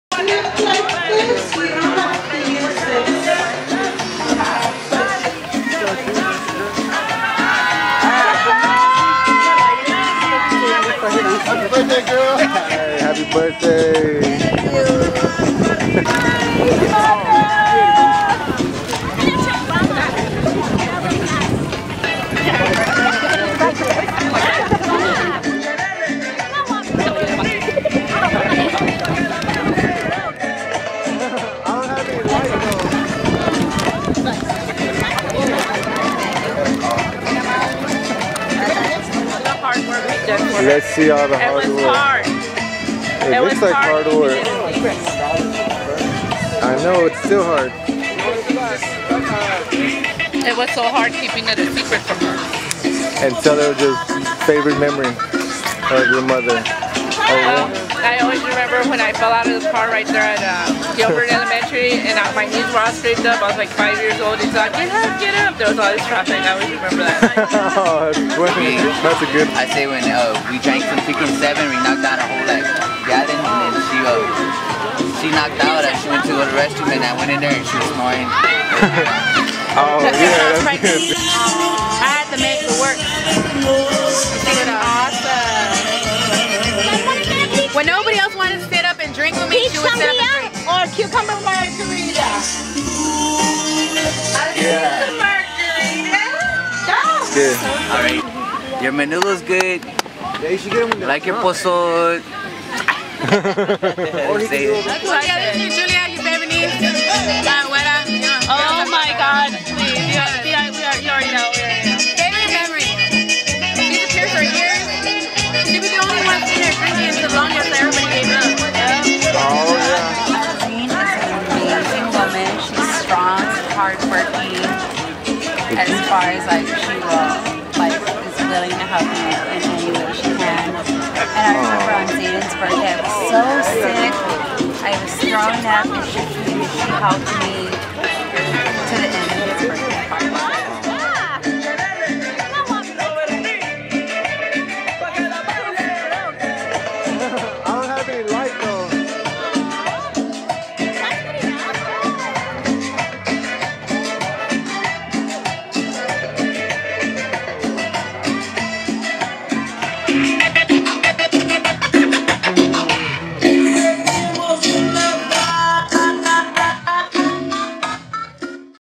Tia Irene
Mini video of  Tia Irene's Bday Party put together by her lovely daughters, Monica, Veronica, Juanita, Delia, and Corina. It was great "PARTY OF THE YEAR"